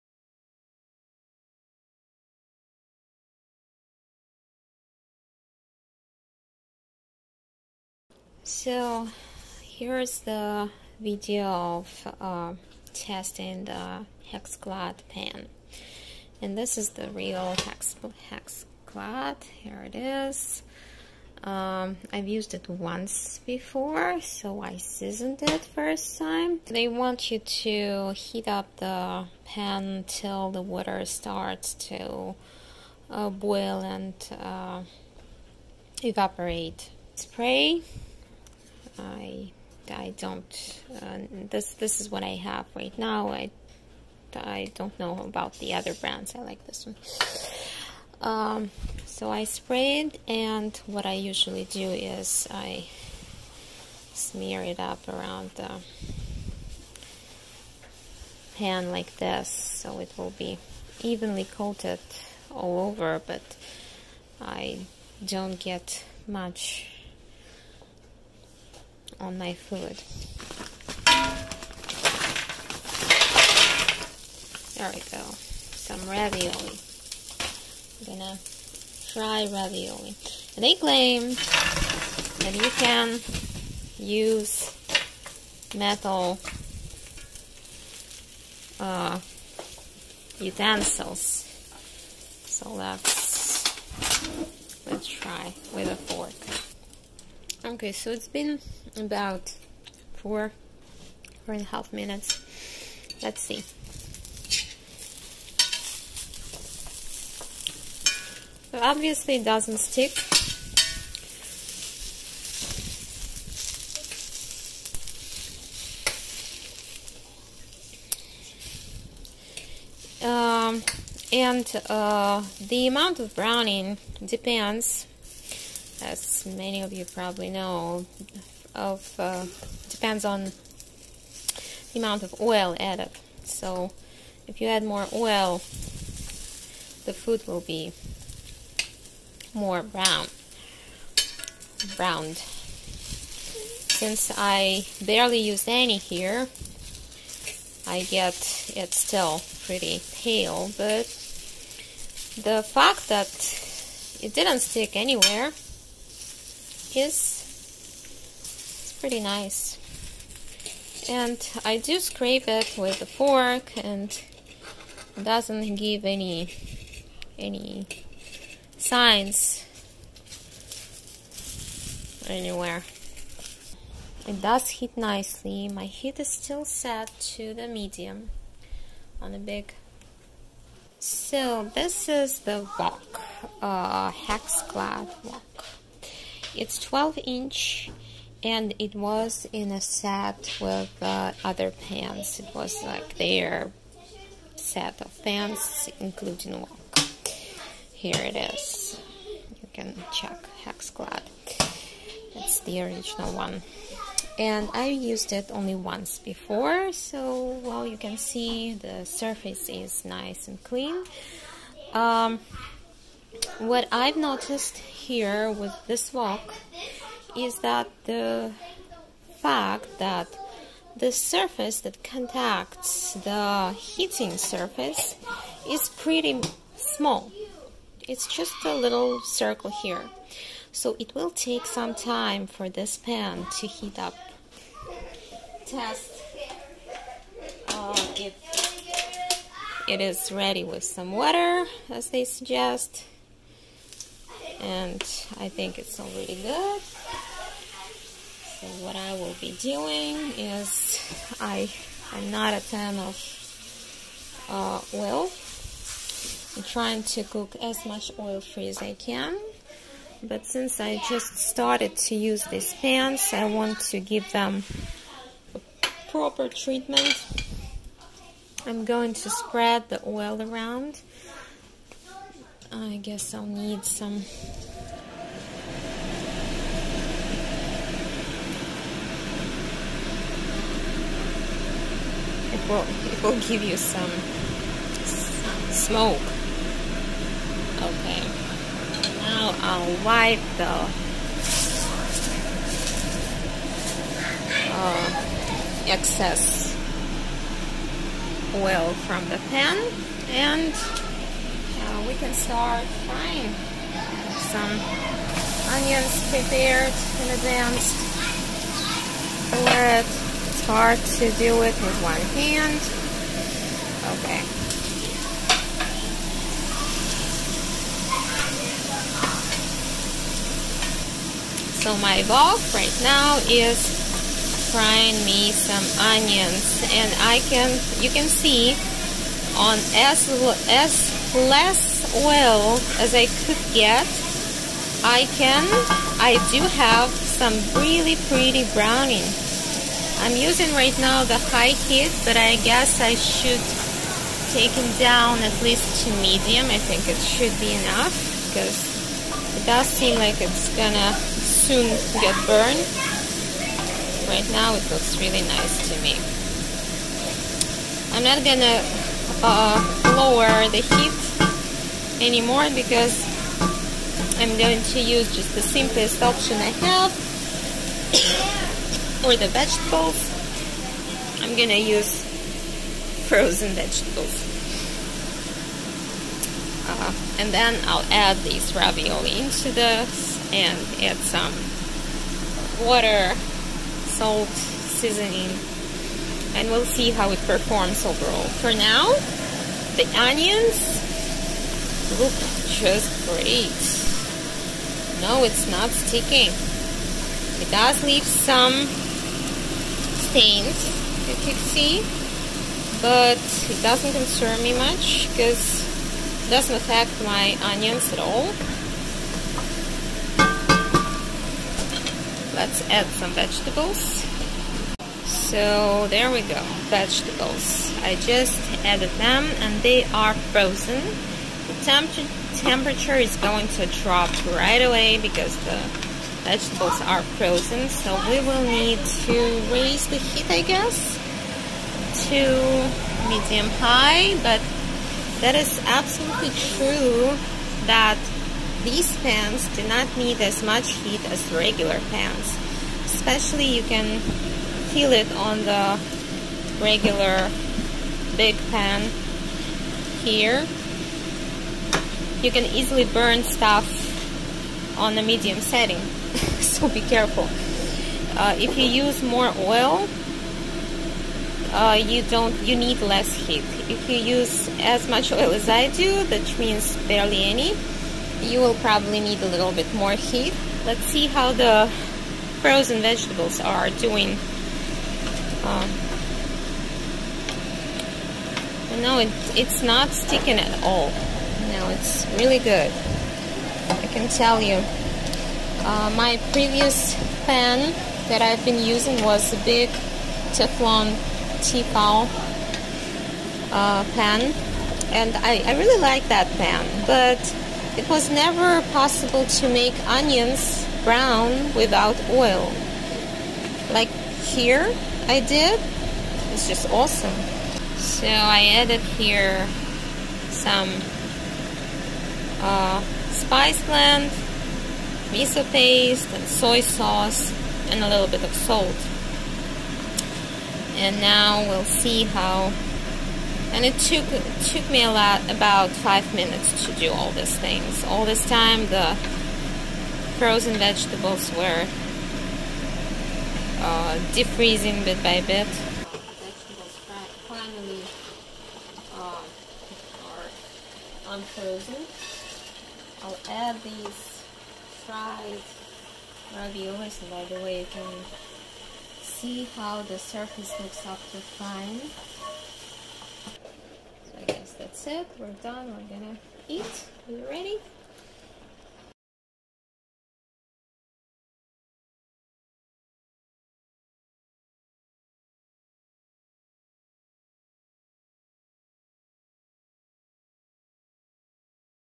HexClad Pan and Wok Test
Unprofessional Cooking, trying HexClad Pan and Wok